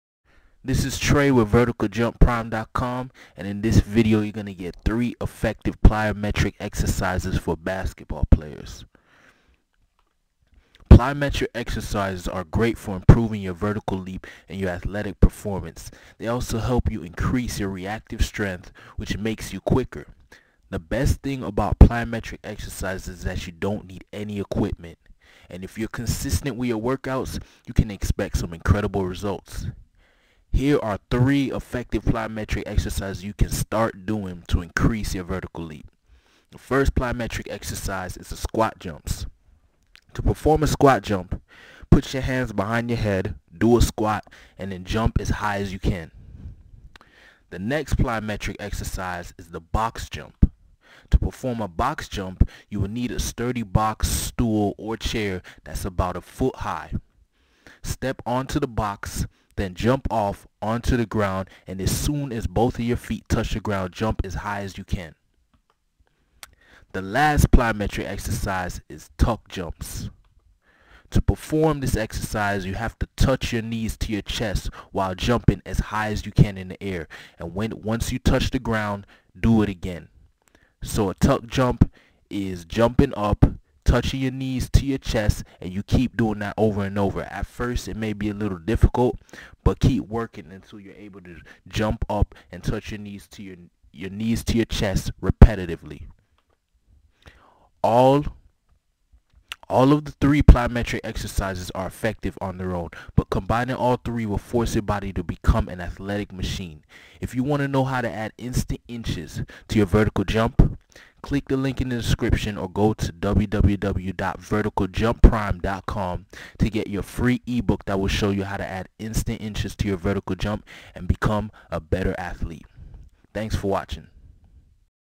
3 Effective Plyometric Exercises For Basketball Players
If you are a basketball player and you want to increase your vertical jump you should be doing plyometric exercises. If you don't know what exercises to do, you will find out three effective exercises in this video.

Plyometric exercises are great because they don't just help you jump higher, they help you move quicker, and make you stronger. Your muscles will become more reactive and that means you will become a better athlete. There are tons of plyometric exercises, but the three exercises in this video are great for basketball players.